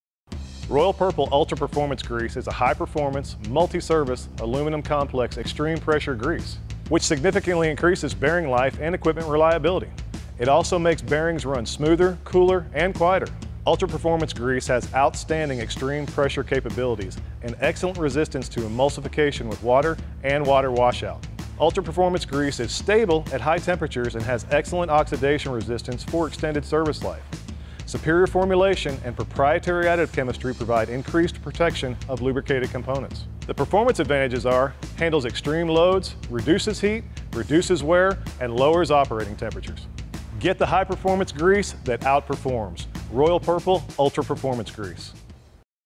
Royal Purple Ultra Performance Grease
Royal Purple UPG is a high performance, multi-purpose, aluminum complex, synthetic EP grease. UPG significantly increases bearing life and equipment reliability.